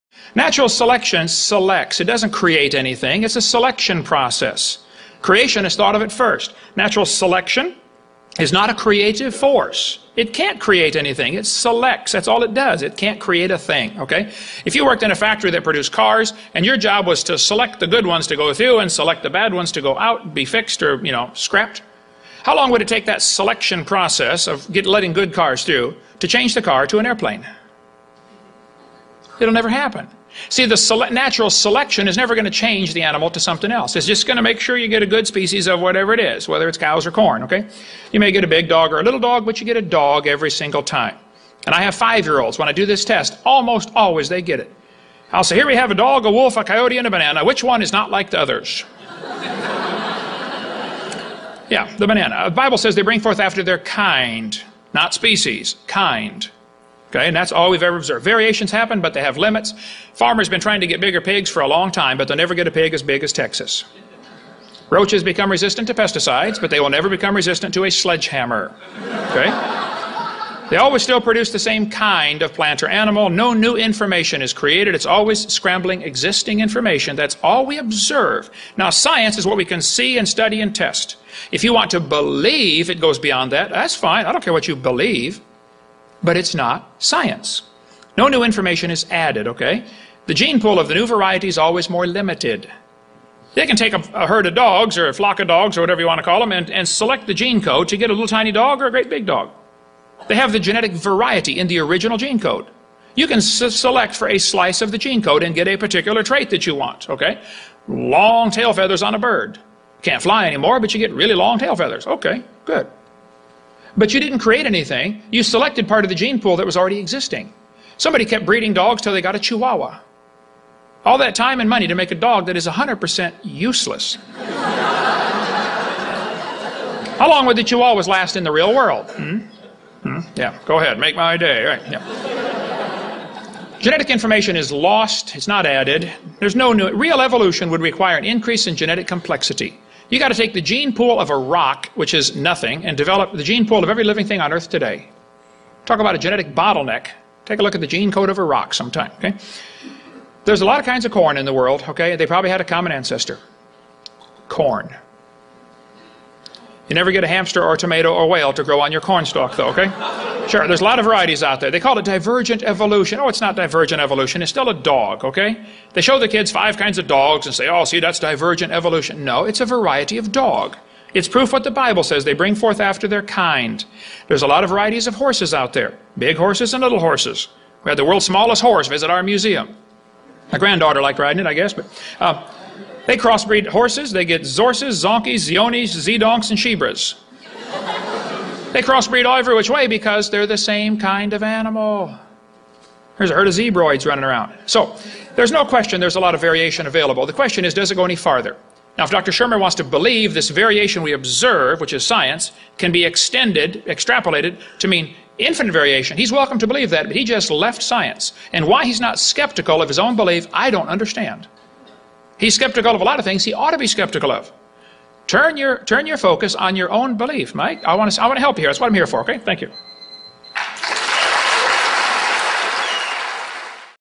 Does NATURAL SELECTION prove evolution? - Kent Hovind
Kent Hovind - Does NATURAL SELECTION prove evolution? (answer to all atheists).

Does natural selection adds new information?